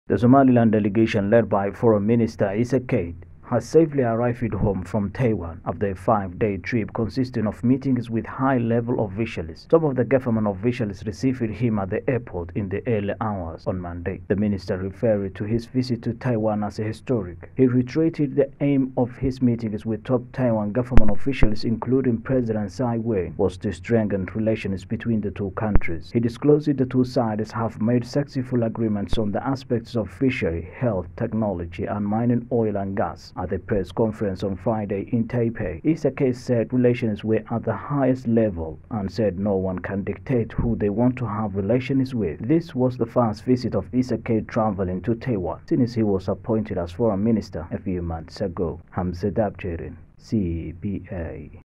Somaliland foreign affairs minister returns home from Taiwan
After a five days trip in Taiwan, Somaliland’s ministerial delegation returned home and received a warm welcome from government officials. This was the first visit Essa Kayd travelled to Taiwan since he was appointed as Somaliland foreign minister………..